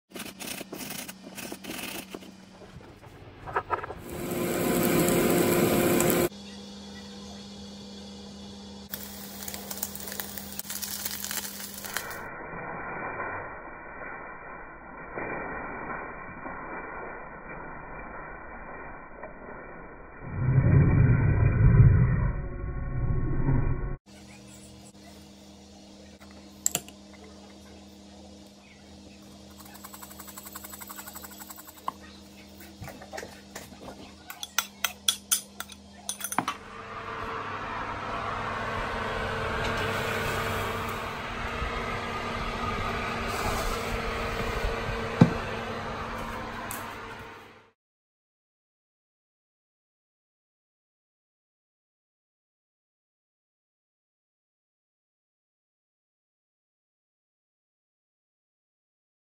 Gluten Free Egg Bacon Sausage Potato Breakfast Muffin
- 3 grated potatoes, fried till crispy
- fried bacon/ham/spam till crispy
- cheddar cheese to taste
- pork sausage
- 6 eggs
- season to taste - I used garlic powder, onion flakes, chilli flakes and salt
- mix and put in muffin pan
Bake 160c fan force or 180c no fan for 15 mins